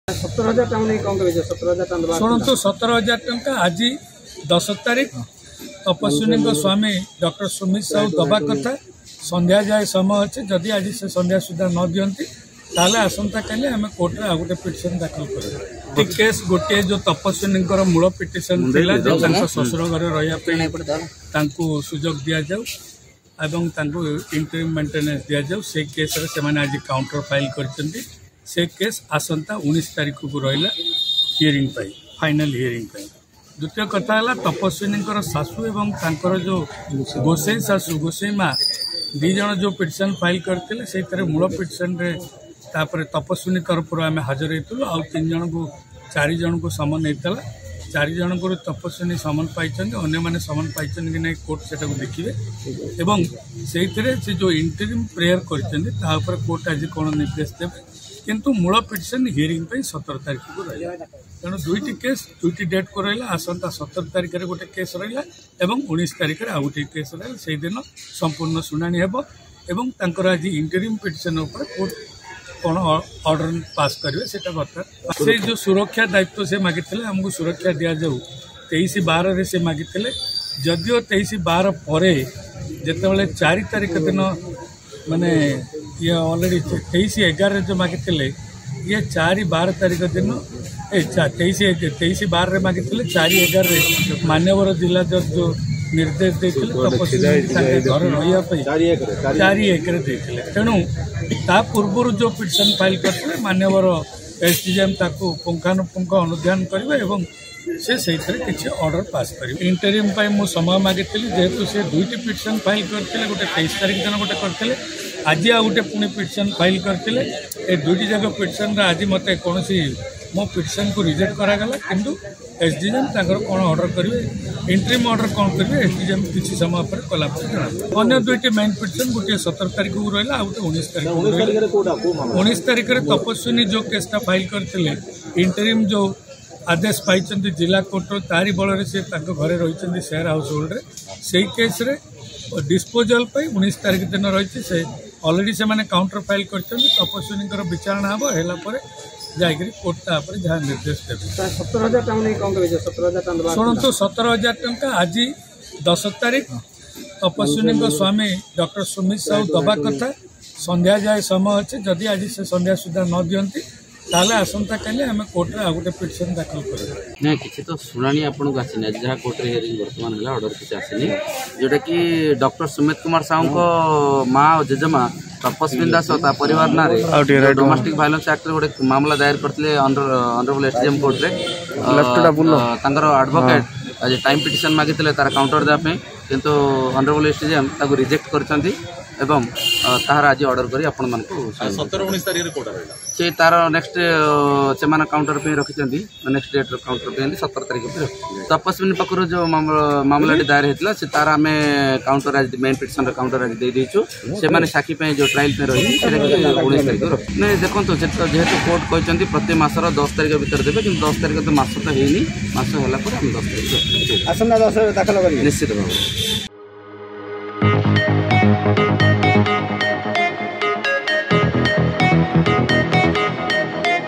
'ସୁମିତ ଦେଲେନି 17 ହଜାର’ | Odisha Reporter
ଜିଲ୍ଲାକୋର୍ଟ ସୁମିତଙ୍କୁ ମାସିକ 17 ହଜାର ଟଙ୍କା ତପସ୍ବନୀଙ୍କୁ ଦେବା ପାଇଁ ଅର୍ଡର ଦେଇଥିଲେ। ସେନେଇ ଣୁଣନ୍ତୁ ଦୁଇ ଓକିଲଙ୍କ ପ୍ରତିକ୍ରିୟା...